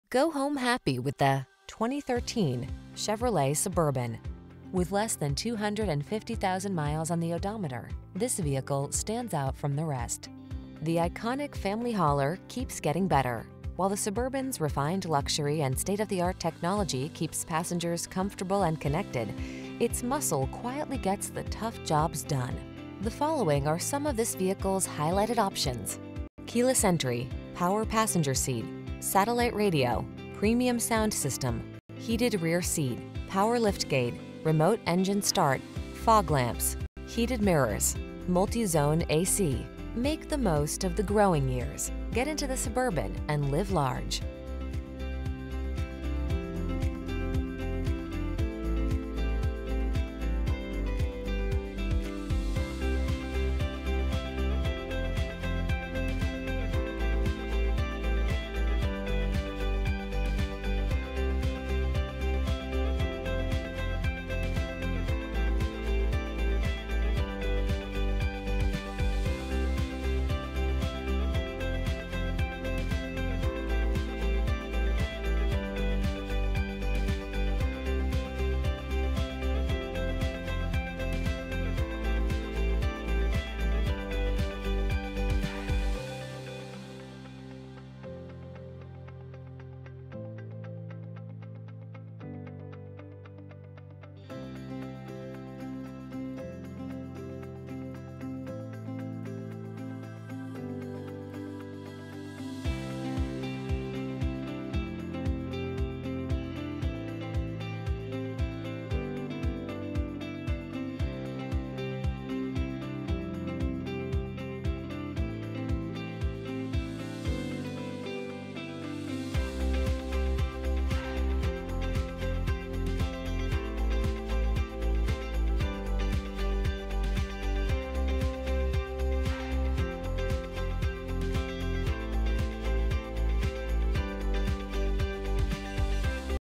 2013 Chevrolet Suburban_1500 McGregor, Waco, College Station, Austin, Kileen CC407A
Summit White Used 2013 Chevrolet Suburban_1500 LT available in 1015 E. McGregor Drive, McGregor, Texas at Jim Turner Chevrolet. Servicing the McGregor, Waco, College Station, Austin, Kileen, TX area.